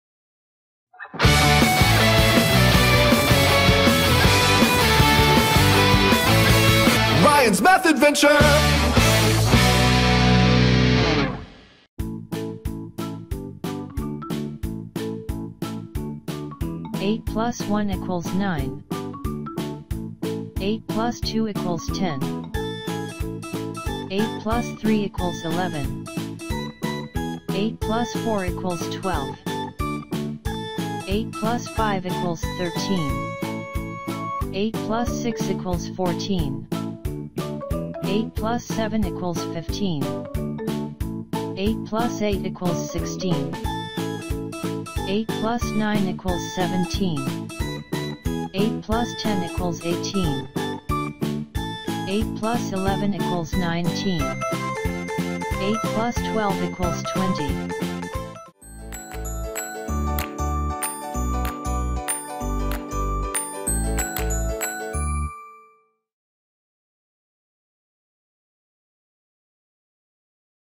Learning Math | Let's learn how to Add by 8
📚Let's learn how to addition with Ryan's friends!
This is table of 8 to 12 👩‍🏫